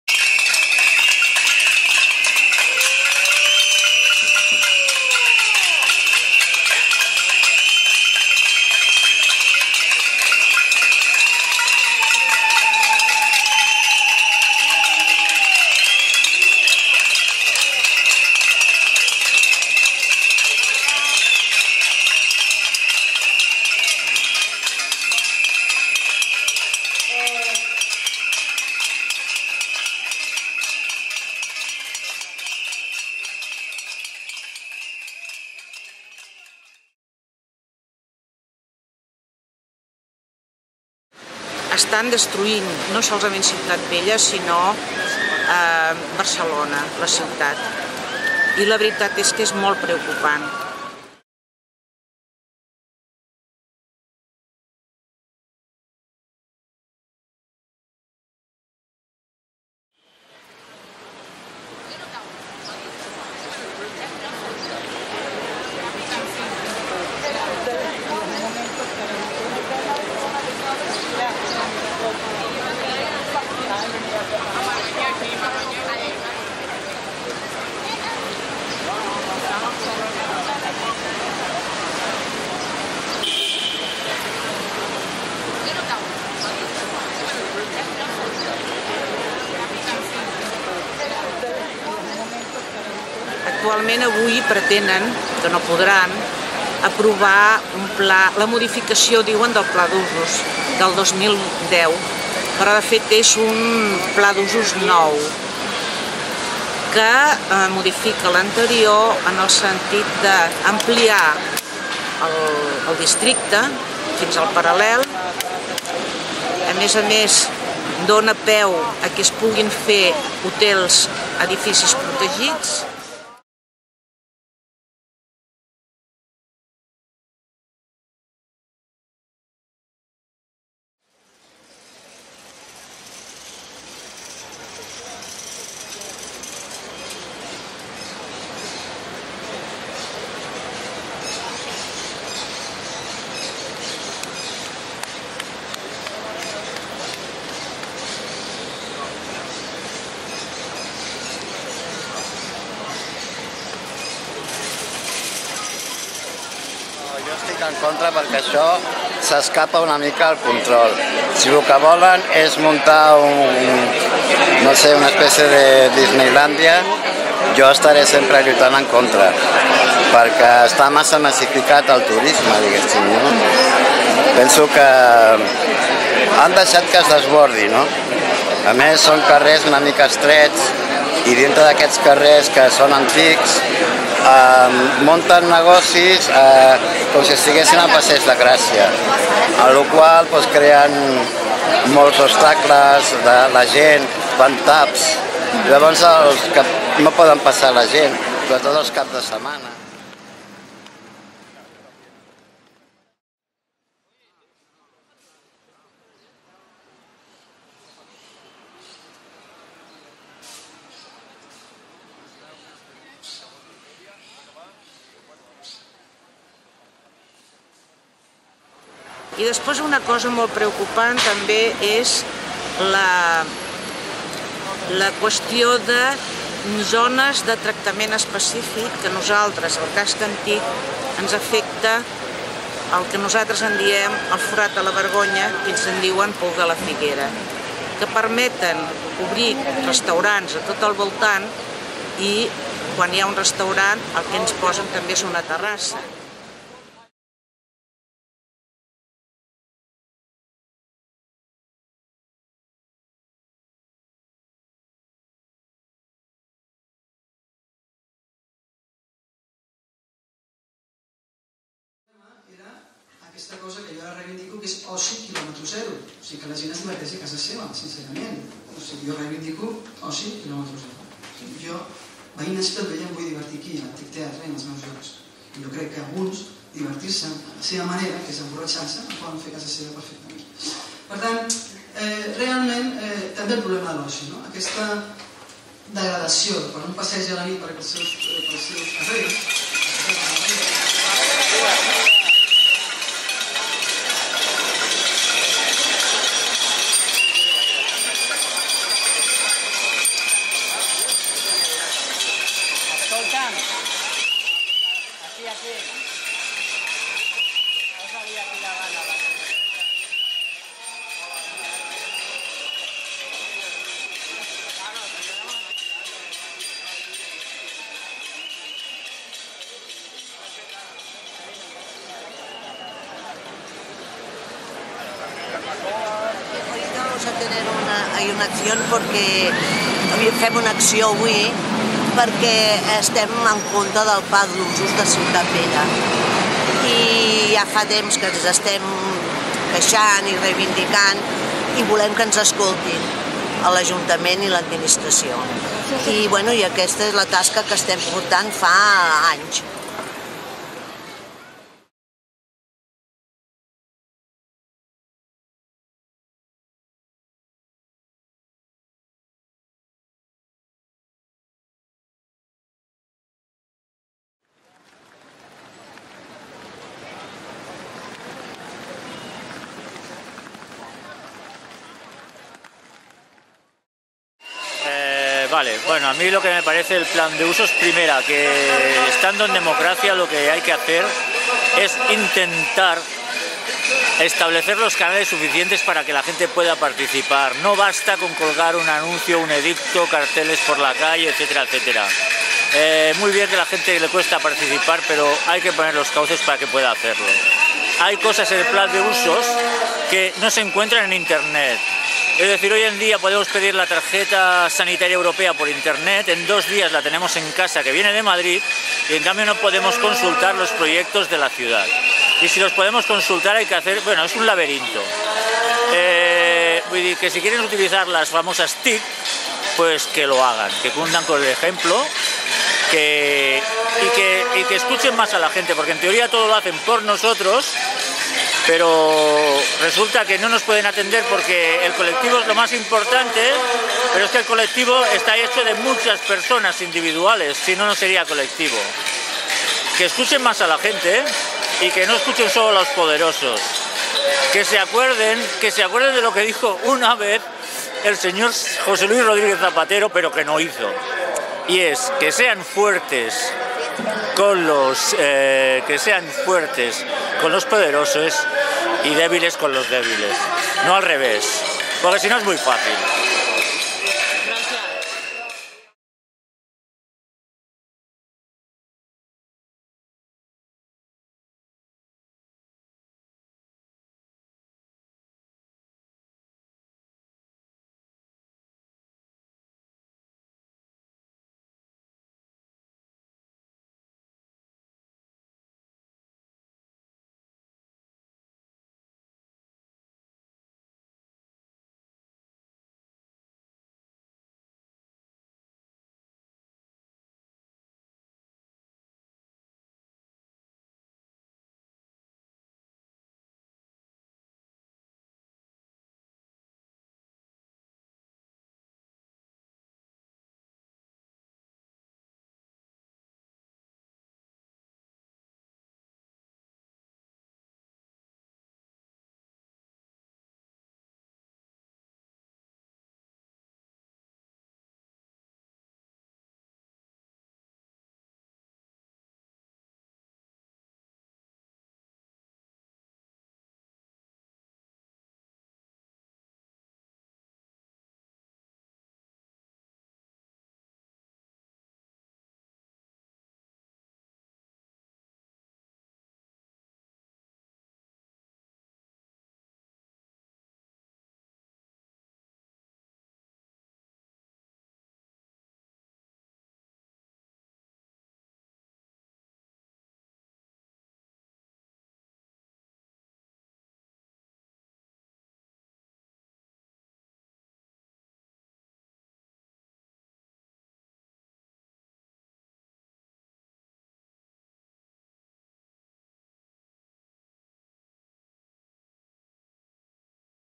Ciutat Vella contra el Pla d'ABusos [Xarxa Veïnal de Ciutat Vella de Barcelona]
Video de la Xarxa Veïnal de Ciutat Vella de Barcelona contra el Pla d'ABusos de l'Ajuntament (aprovat amb els vots de CIU i PP), que permet la construcció de més hotels i pisos turístics als barris de Ciutat Vella (Casc Antic - Sant Pere, Santa Caterina i La Ribera, Gòtic, Raval, Barceloneta)